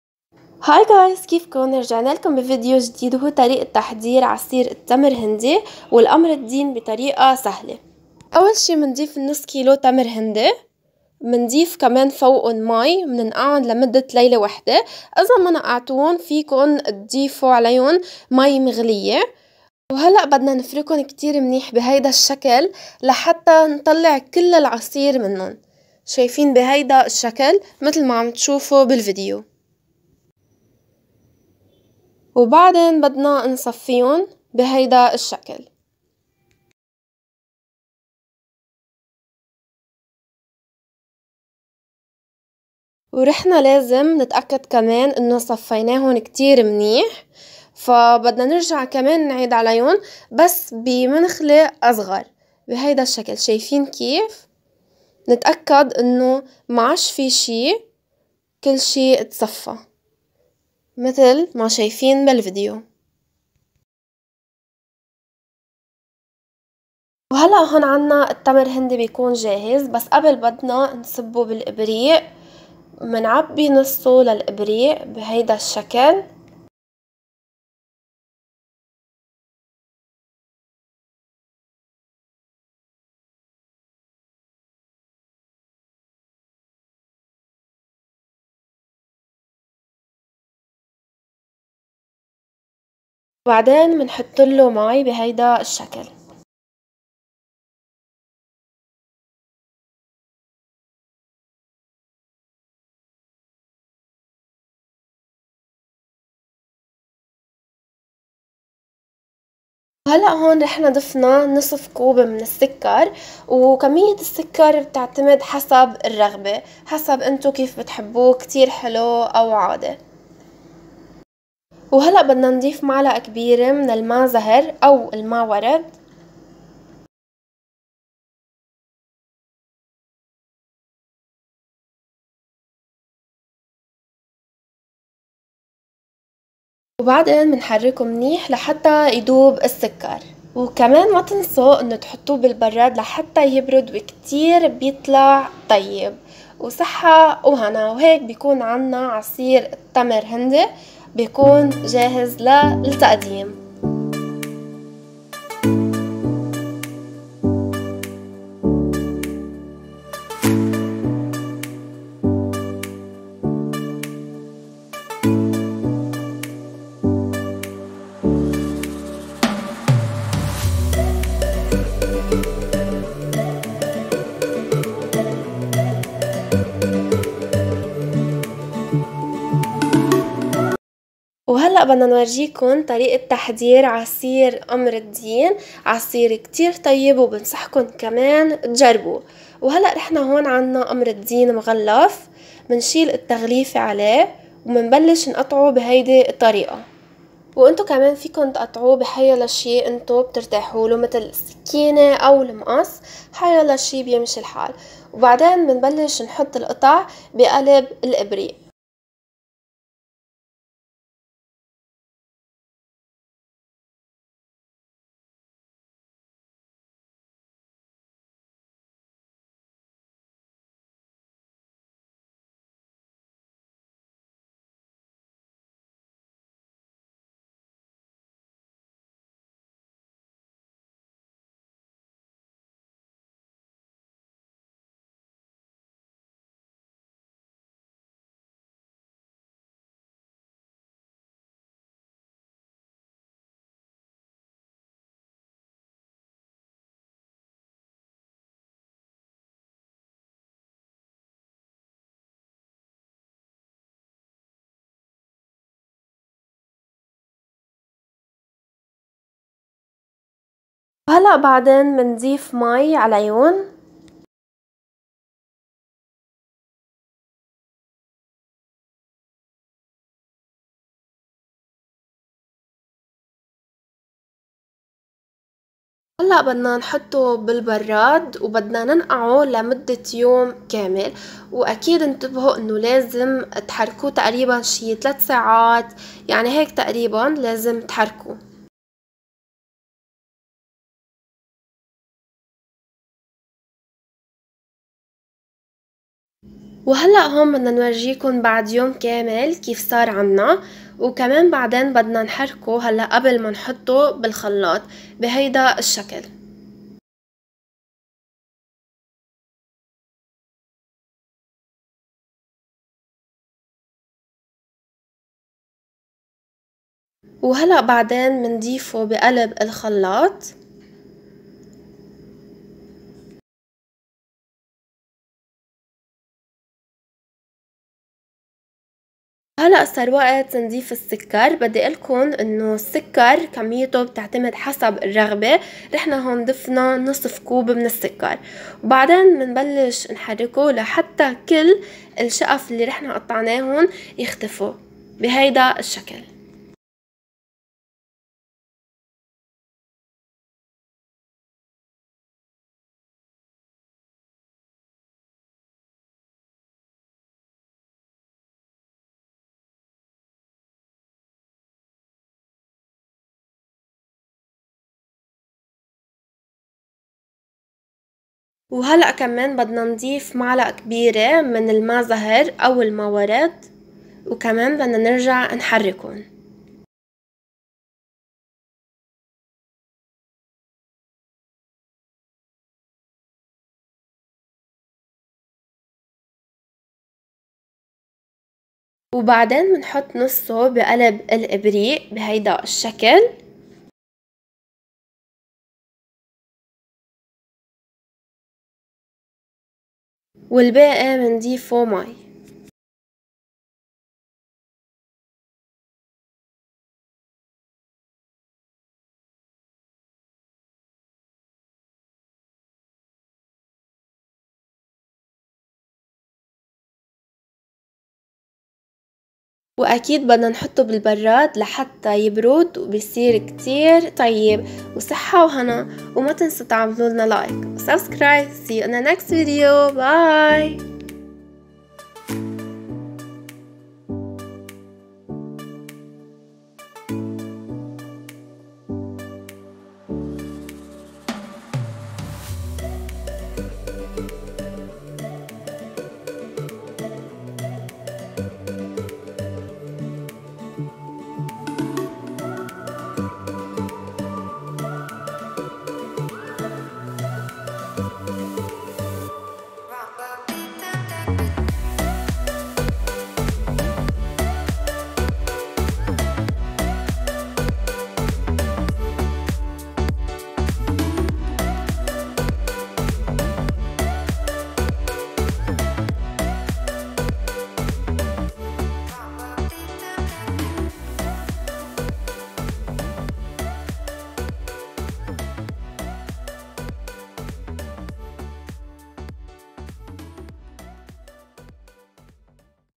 عصائر رمضانية - قمر الدين و تمر هندي
Today we share with you some of the delicious juices served during Ramadan, Tamarind juice and Qamar Al Deen Juice (Dried apricot based). Hope you will enjoy!

اليوم نشارككم بعض العصائر اللذيذة المقدمة في رمضان وهم عصير التمر الهندي وعصير قمر الدين (المشمش المجفف). نأمل أن تستمتعوا!

مرحبًا ،
dirovers ،

:تأكد من الاشتراك معنا ومتابعتنا على حسابات وسائل التواصل الاجتماعي لدينا

رمضان،طريقة تحضير قمر الدين،طريقة تحضير تمر هندي